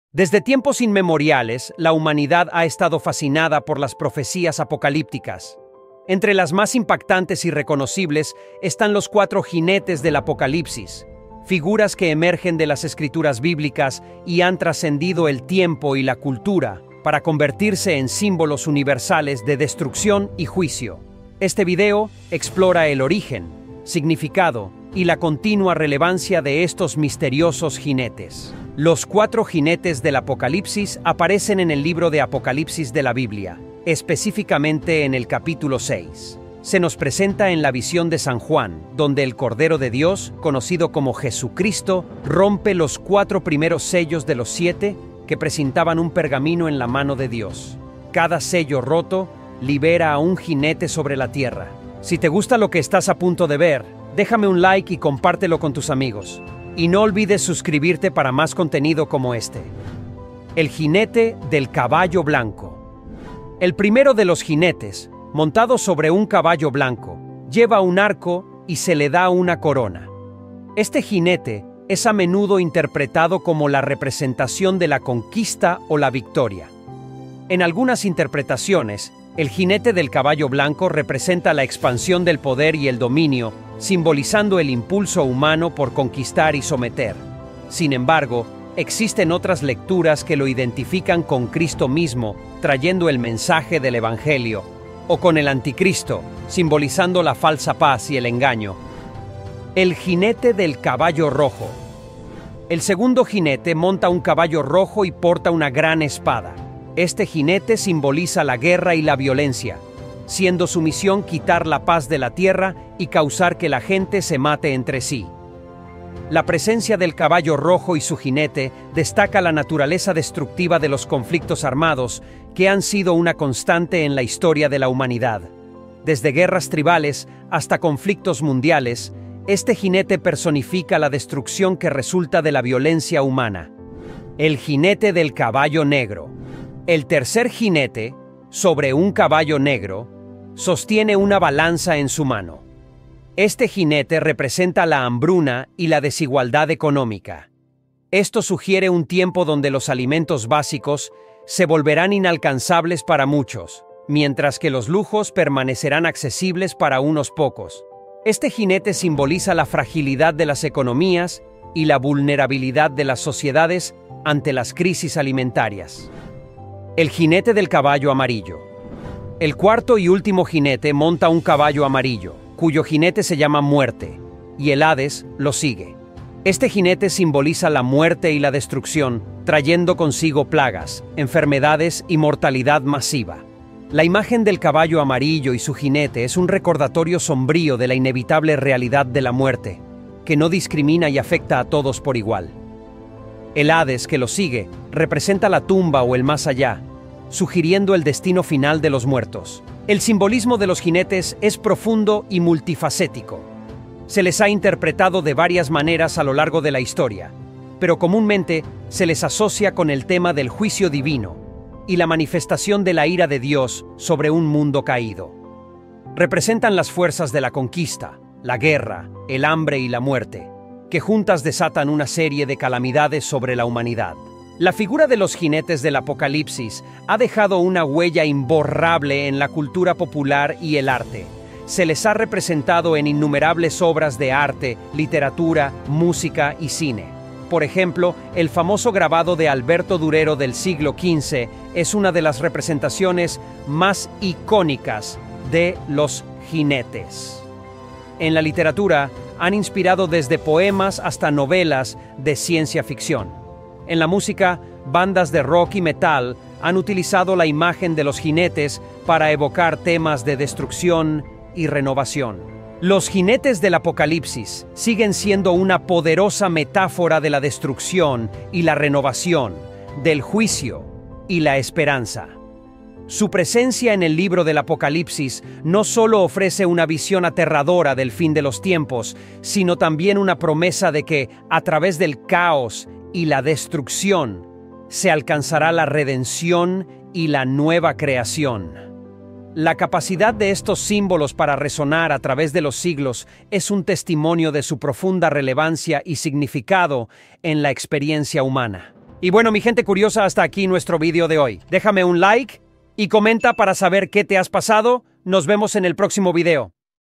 Los JINETES del APOCALIPSIS ¿Profecia o Realidad?
¿Alguna vez te has preguntado qué significan realmente los Cuatro Jinetes del Apocalipsis?. Pues en este video te voy a desvelar el misterio detrás de estas figuras enigmáticas que han capturado la imaginación de millones de personas a lo largo de los siglos.
Desde la guerra y la conquista, hasta el hambre y la muerte, analizaremos cada uno de los jinetes, su simbología y como su influencia se refleja en las profecías bíblicas.
¡Prepárate para un viaje fascinante a través de los textos sagrados y descubre si estos jinetes son un presagio del fin del mundo o algo mas!